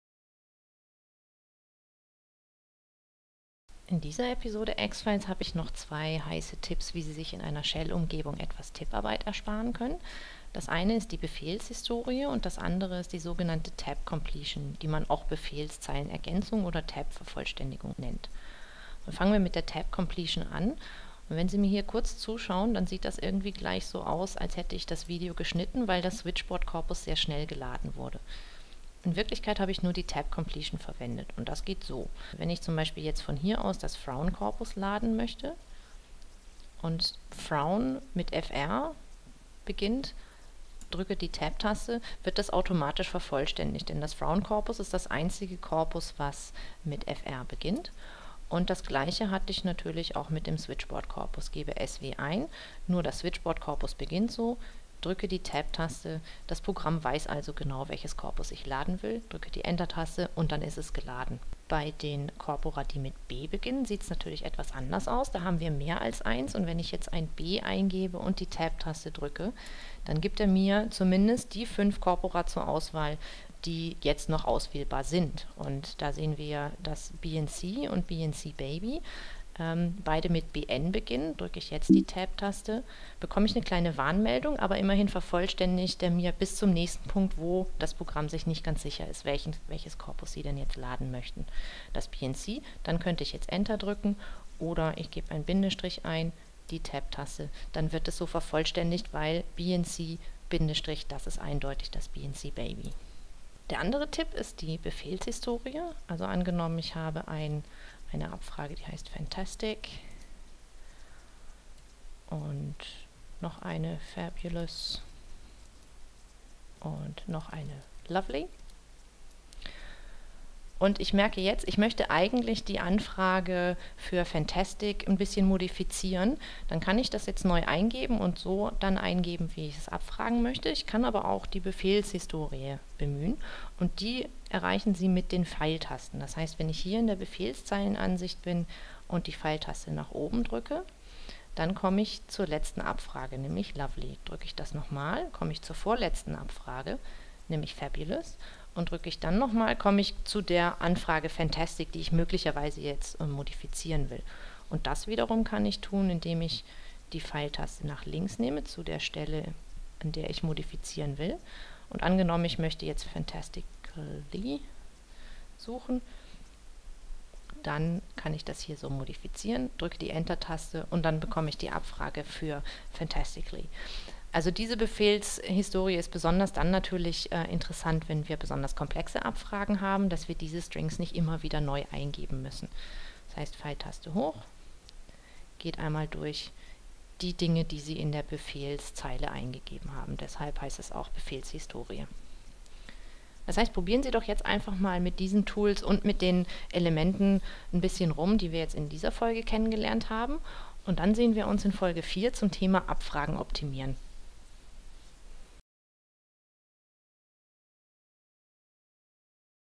Korpustutorial — §3.x: Tipparbeit sparen (X-Files)
Zwei wahnsinnig nützliche Tipps, um sich in einer Shell-Umgebung Tipparbeit zu sparen: Befehlshistorie und Tab-Completion